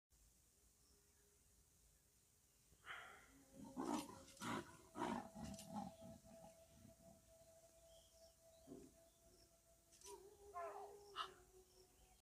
Lioness surprise attack on Leopard On the road.
In this thrilling video, we see a rare and intense moment in the wild as a lioness launches a surprise attack on a leopard. The scene takes place on a road in the African savanna, surrounded by lush vegetation and the sounds of the wilderness. The lioness, known for its strength and hunting skills, sneaks up on the unsuspecting leopard with lightning speed. The leopard, quick to react, tries to defend itself but is overpowered by the lioness. The two big cats engage in a fierce battle, rolling and grappling with each other as they try to gain the upper hand. Their growls and roars fill the air, making it clear that this is a life or death struggle. The intense action is captured in detail, giving viewers an up-close look at the raw power and natural instincts of these magnificent predators. This video is sure to leave you on the edge of your seat, and with a newfound appreciation for the incredible animals that roam the African savanna.